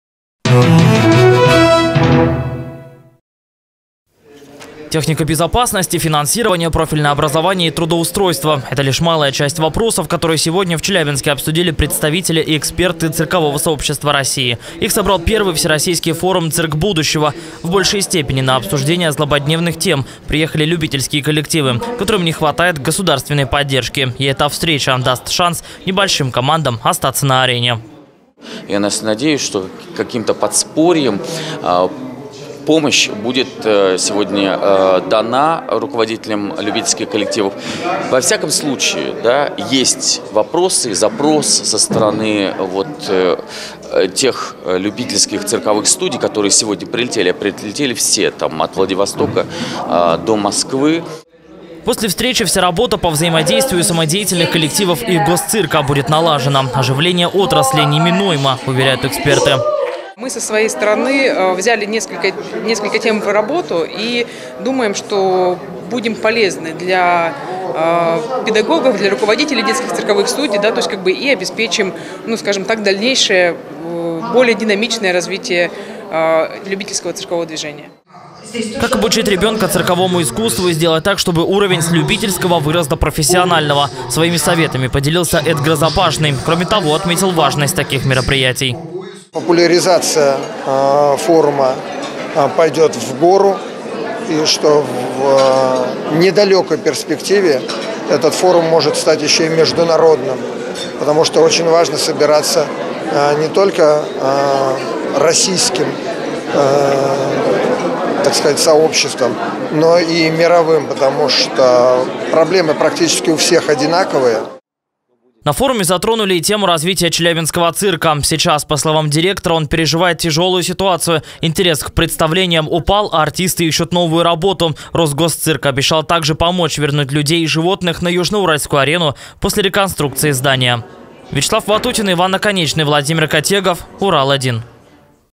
В Челябинске собрались представители и эксперты циркового сообщества России
Эта встреча даст шанс небольшим командам остаться на арене